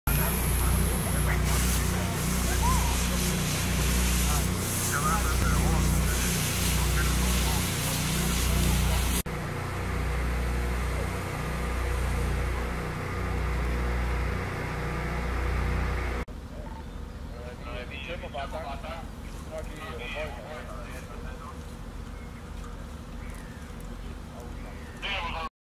Naturbrand i Herning
14.04.21: Brandvæsenwt måttenonsdag slukke naturbrand ved Herning